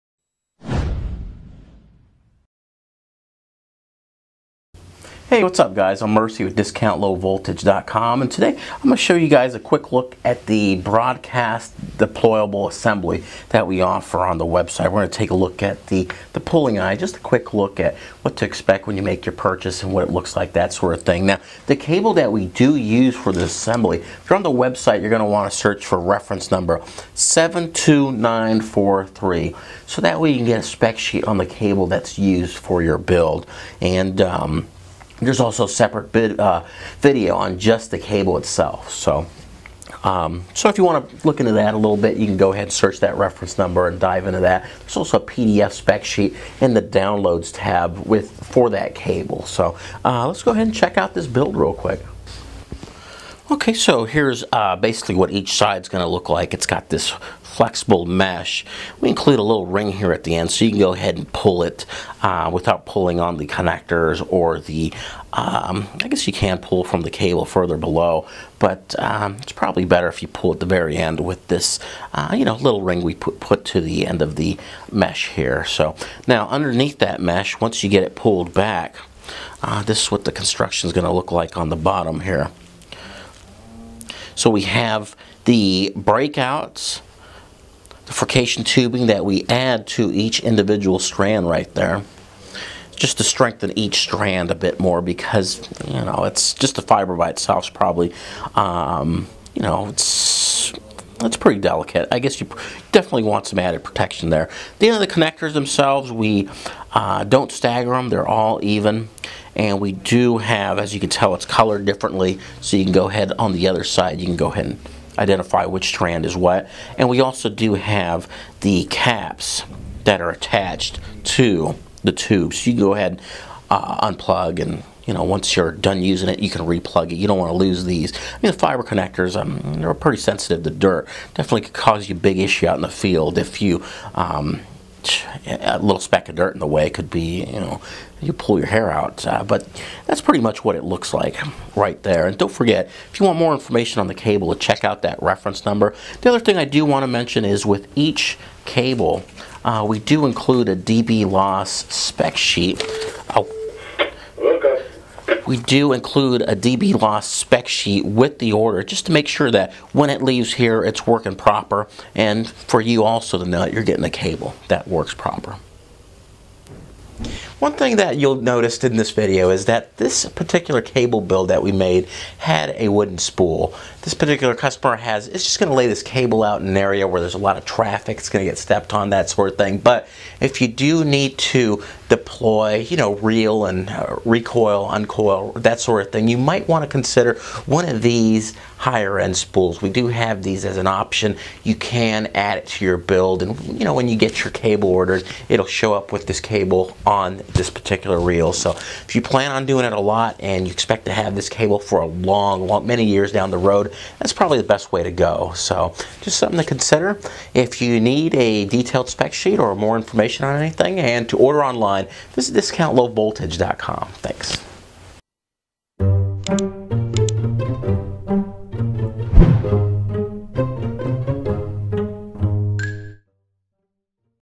Fiber Optic Cable for Broadcast Applications and Installations
Here's a great look at one of our many different assemblies. This assembly is used with a broadcast/tactical fiber optic cable and is ideal for use in installations where extreme environmental conditions are present.  Designed to be deployed and retrieved in the field, Tactical/Broadcast Tight Buffered Cables are highly resistant to damage caused by repeated impacts or harsh conditions.

These assemblies can be made per your requirement.